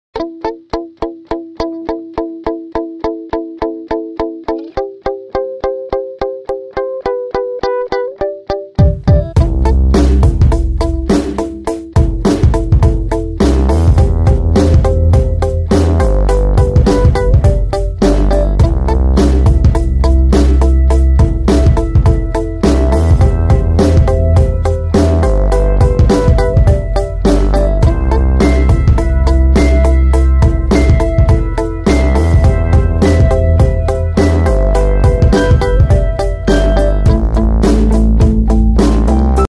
How To Use ZS-XG440C Automatic Ropp Capping Machine With Cap Feeder
1. We can customize multiple capping head according to operation need.
2. It can work with automatic filling machine and labeling machine to realize automated production.